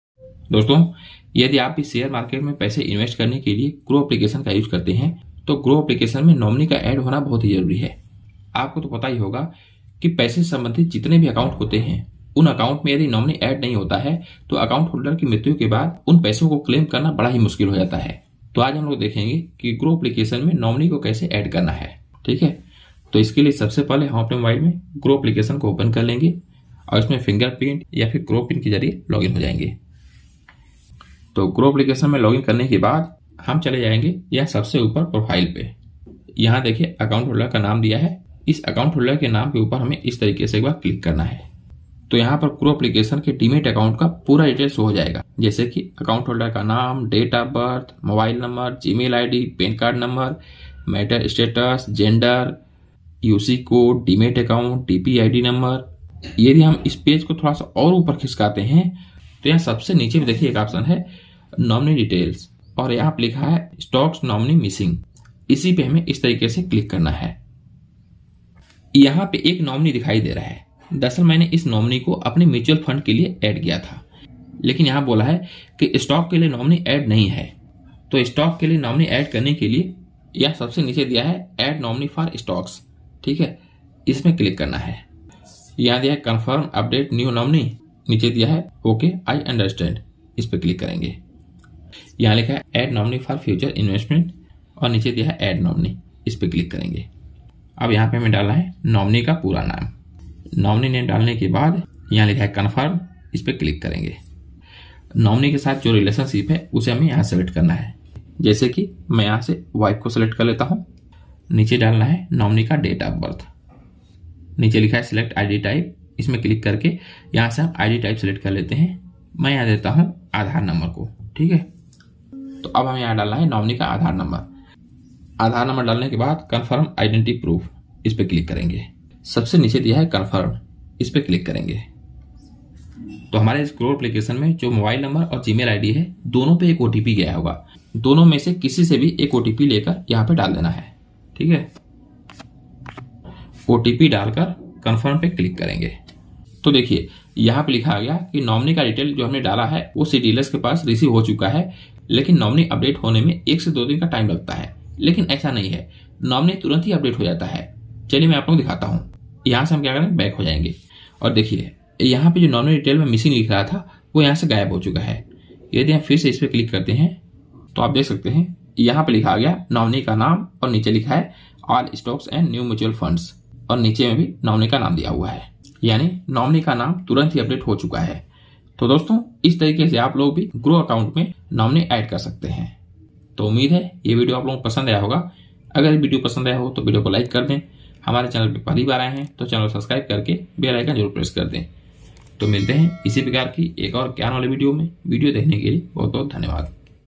How to Add Nominee in Groww app | Groww में नॉमिनी जोड़ना सीखें | Nominee link in Groww app
is video me maine groww app me nominee add karne ka tarika bataya hai, isi tarah ki aur bhi video dekhne ke liye mere is channel ss tech knowledge ko subscribe jarur kar lijiye.